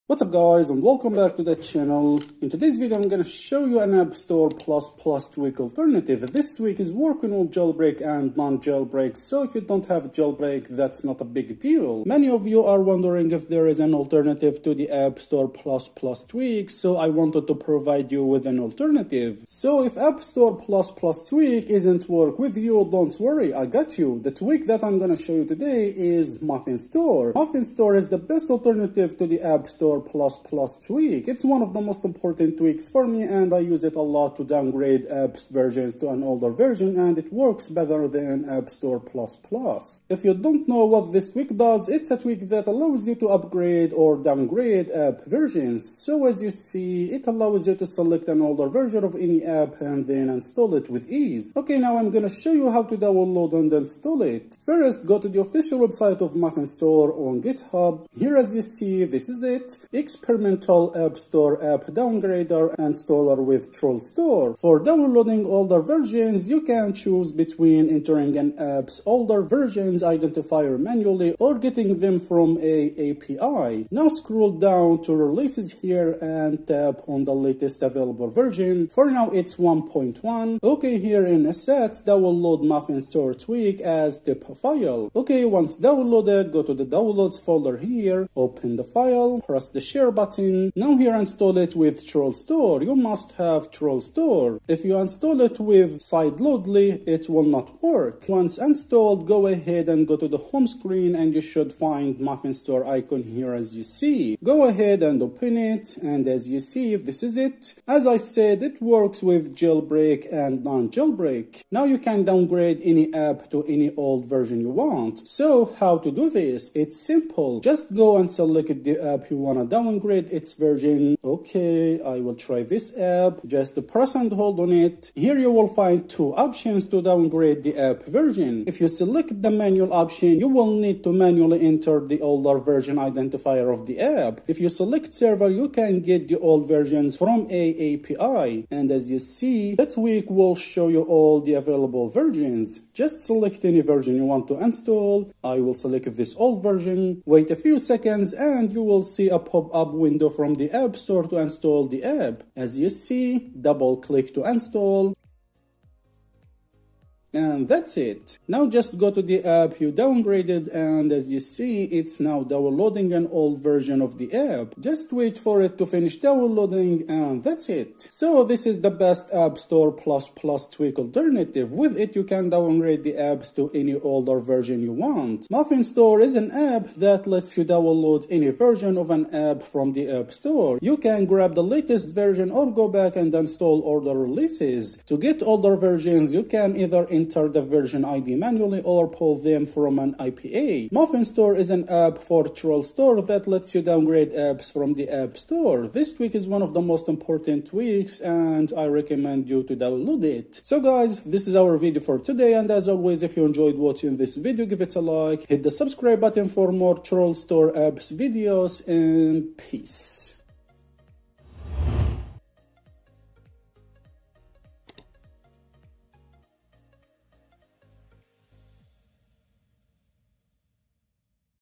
The BEST AppStore++ Tweak Alternative! Downgrade ANY App Version!
🚀 MuffinStore is the BEST AppStore++ alternative for downgrading ANY app version on iOS – working on both jailbroken and non-jailbroken devices! In this video, I’ll show you how to install and use MuffinStore with TrollStore to easily downgrade apps to older versions.

🔹 Why MuffinStore?
✔️ Works without jailbreak (TrollStore required)
✔️ Lets you downgrade ANY app version
✔️ Two methods: Manual ID entry or API-based selection
✔️ Better than AppStore++ – More reliable & updated!

🔧 Tutorial Steps:
1️⃣ Download MuffinStore .Tipa from GitHub
2️⃣ Install via TrollStore (Sideloadly won’t work!)
3️⃣ Open MuffinStore & select your app
4️⃣ Choose an older version & install!

More 🙂
My eBay store
✮═══ஜஜ🔆💜🔆ஜஜ═══✮
★═ஜ💠ஜ═✮